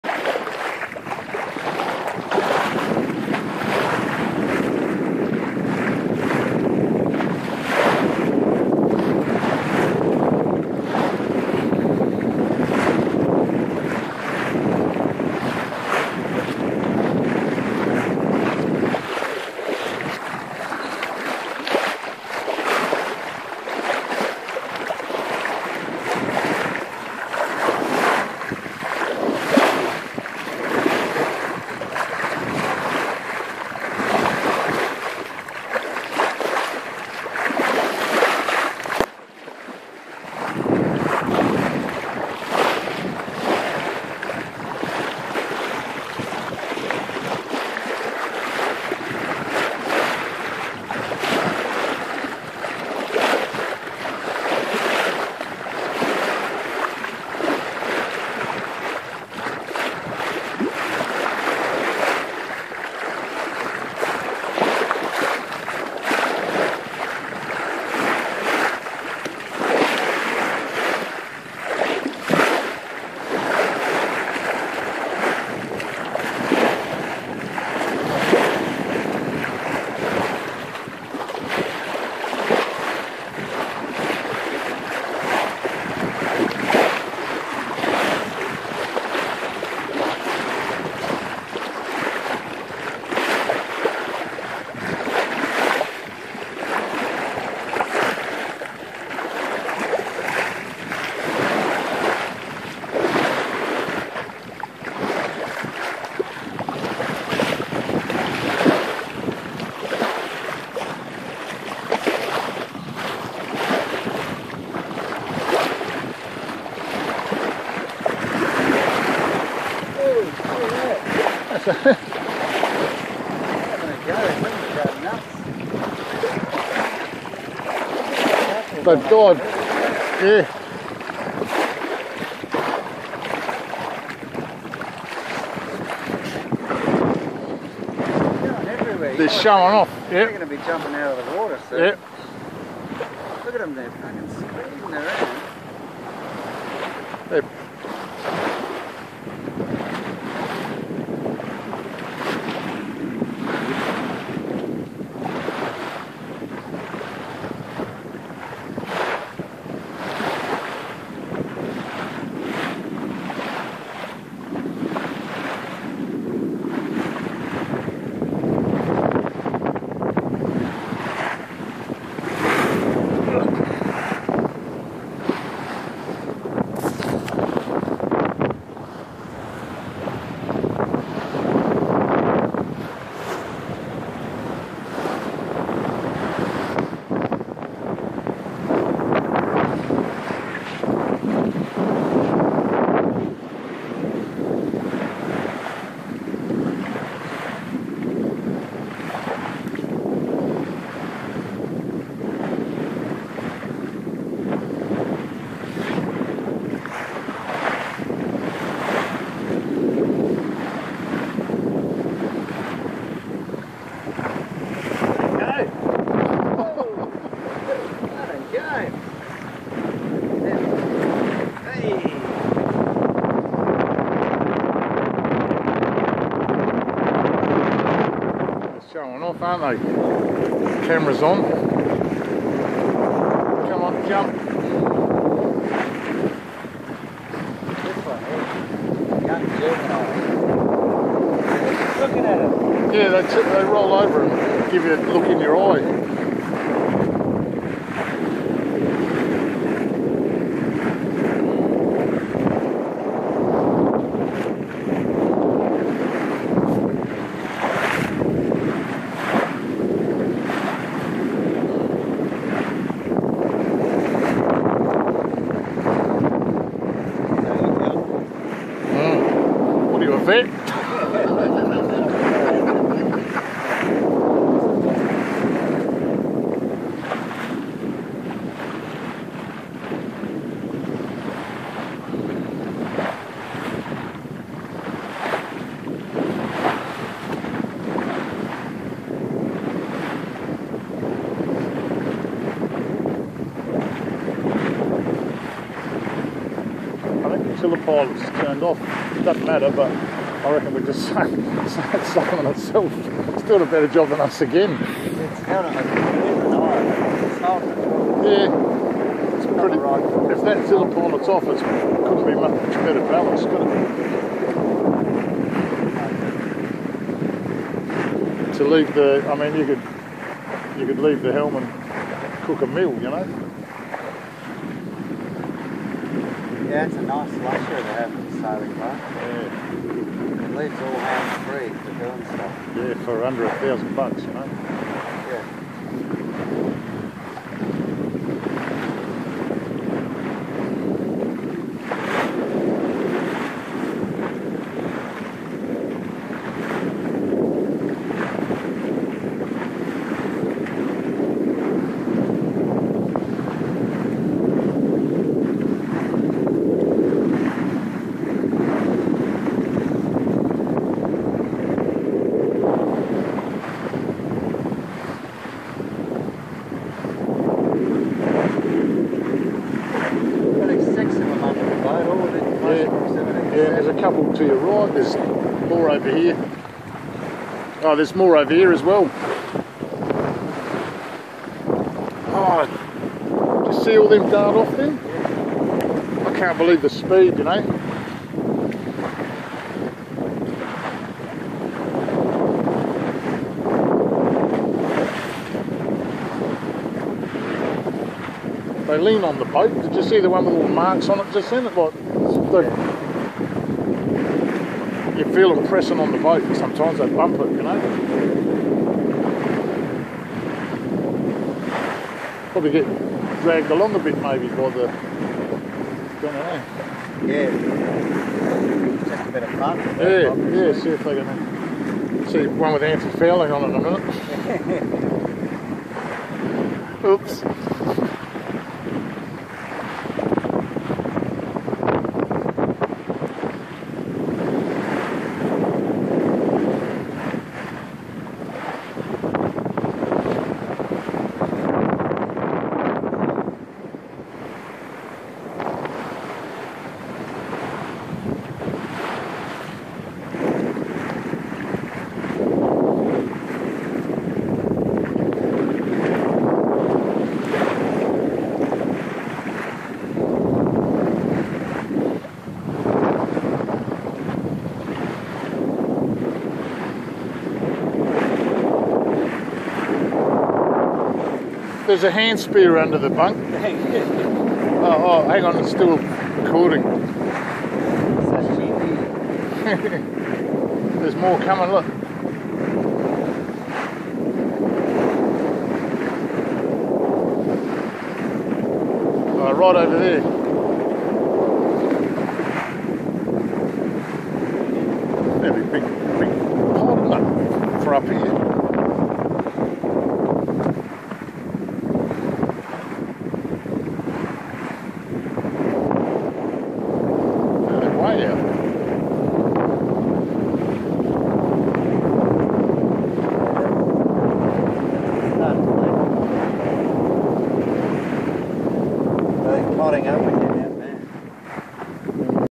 Tahiti Ketch Pelorus crossing the gulf
Coming back from Port Vincent after a club cruise. Wind is around ten knots and tiller pilot is steering us to the black pole! Sailing is hard work.